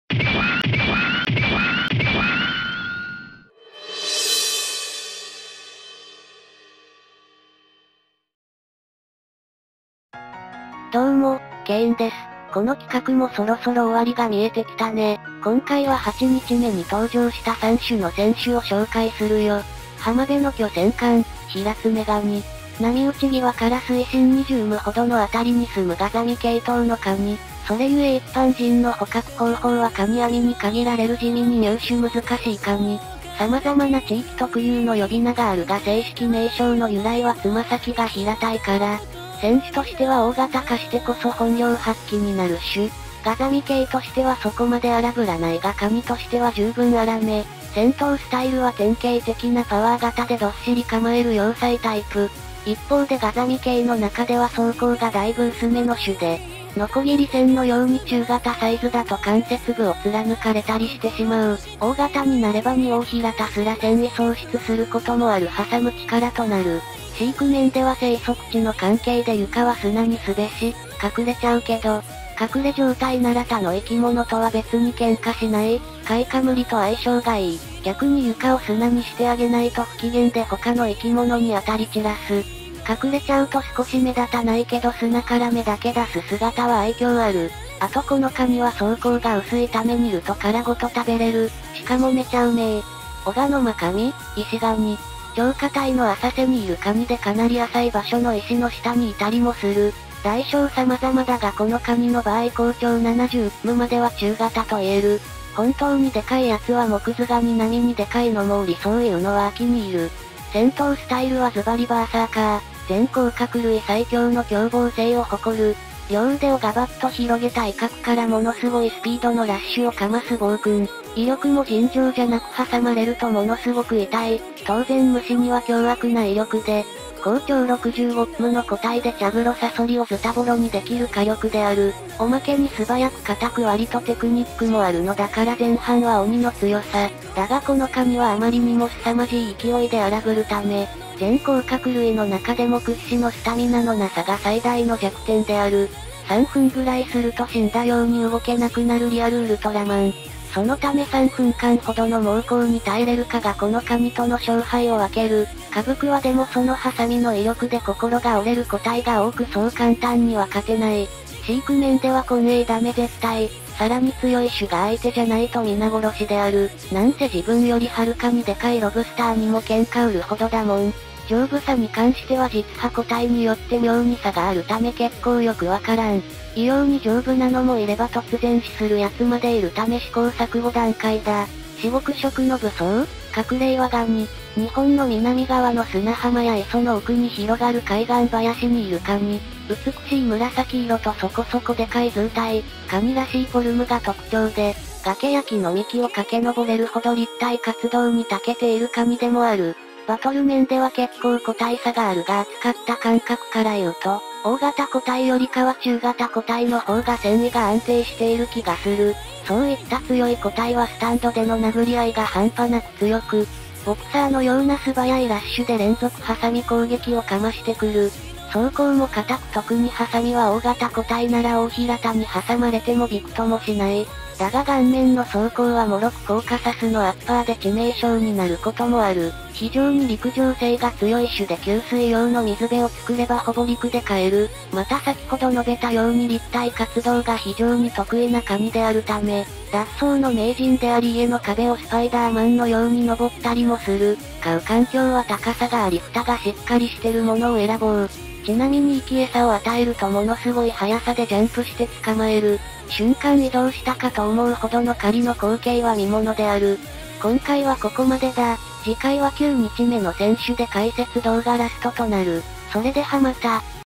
【甲殻類】ビートルレジスタンス出場選手軽めの紹介パート７～ヒラツメガニ、イシガニ、カクレイワガニ～
ビートルレジスタンスに登場した甲殻類紹介動画パート７
今回紹介するのは
ヒラツメガニ
イシガニ
カクレイワガニ
となります。戦いもいよいよ終盤。次回で紹介動画は最後となります。